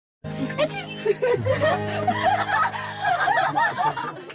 TWICE's *REACTION* when Nayeon did an aegyo (ACHOO!) so funny!
Watch Nayeon do the achoo ft. TWICE